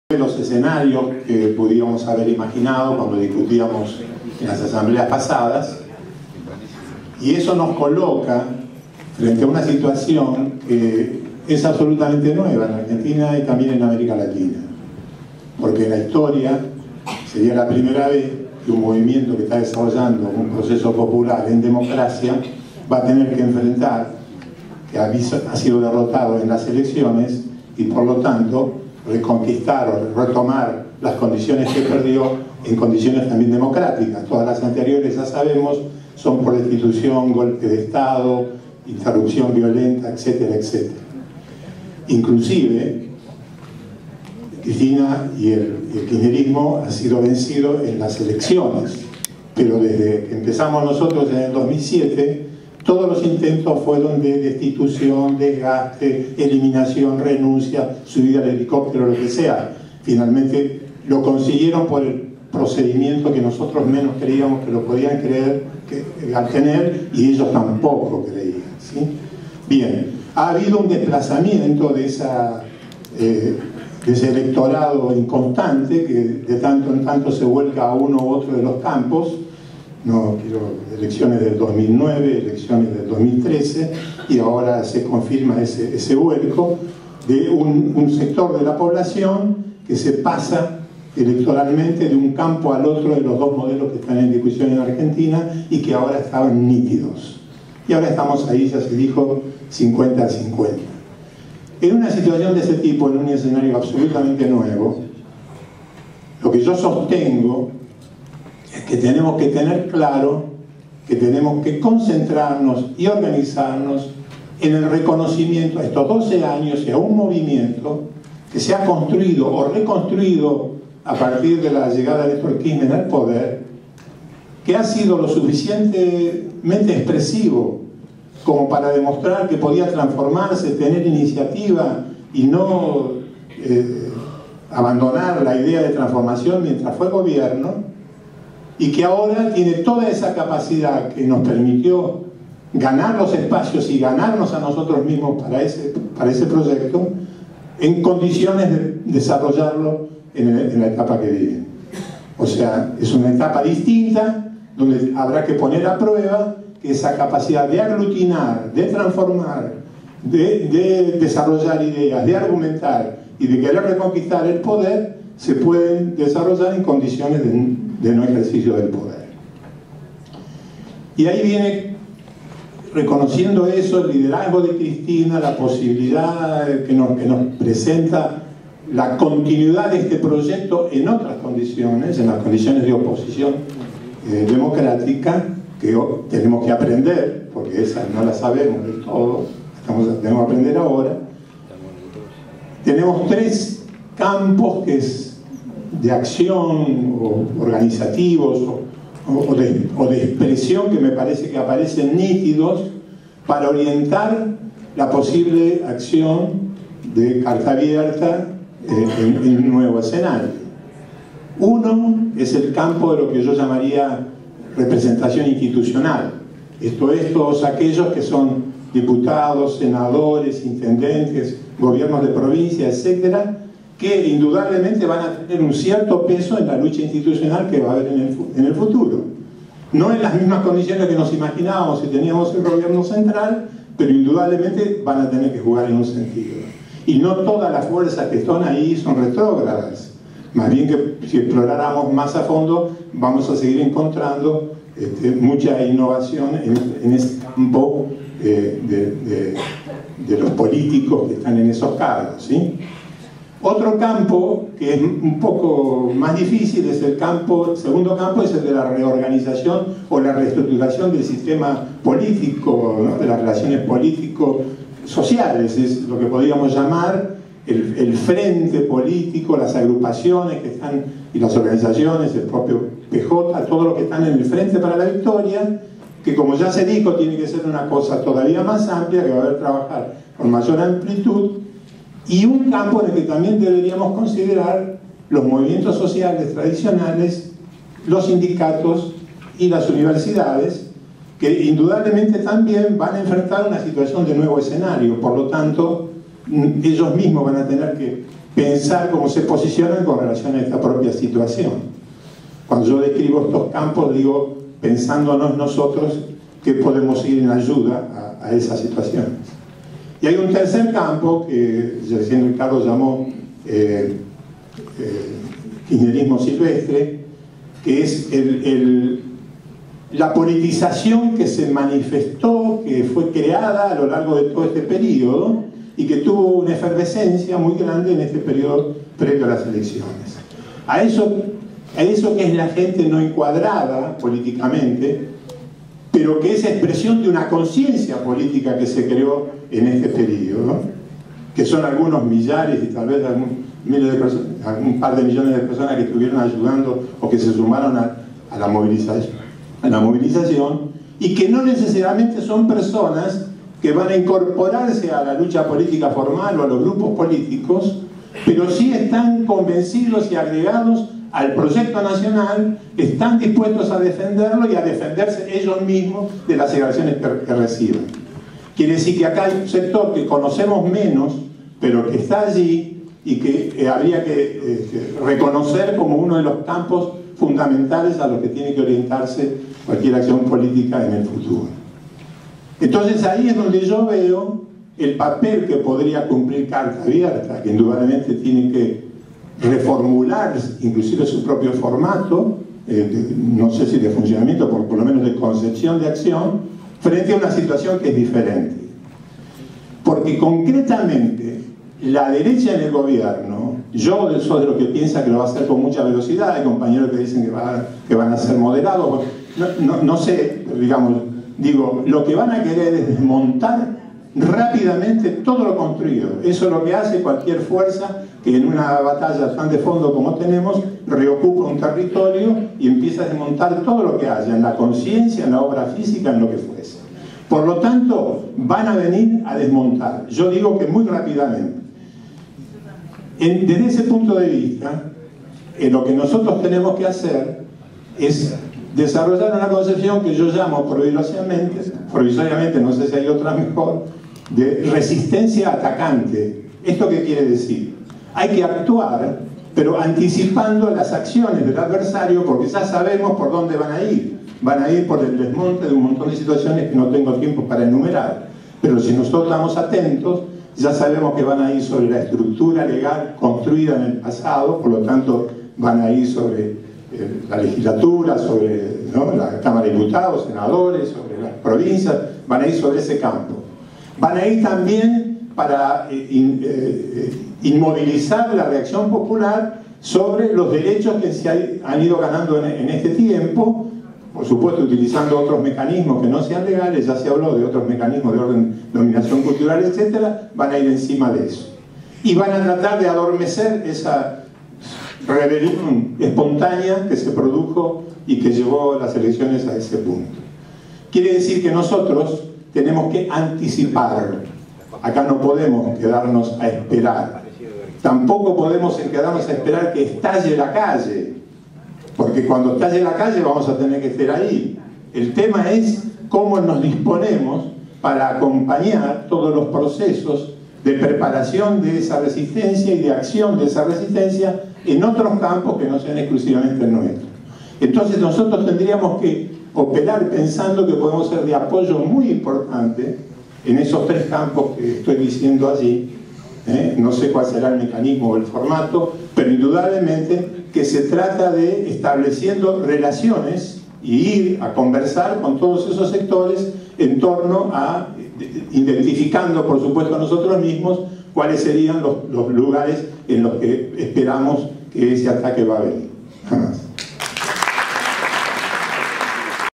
14CA Hector Poggiese 281115
Asamblea de Carta Abierta del sábado 28 de noviembre de 2015. Biblioteca Nacional. Buenos Aires.